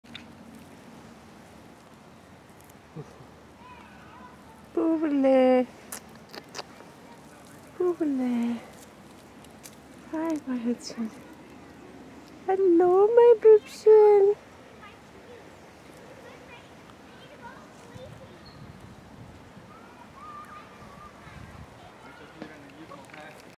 Beacon Hill Park Victoria BC
Park in mid-November Peacocks and Ducks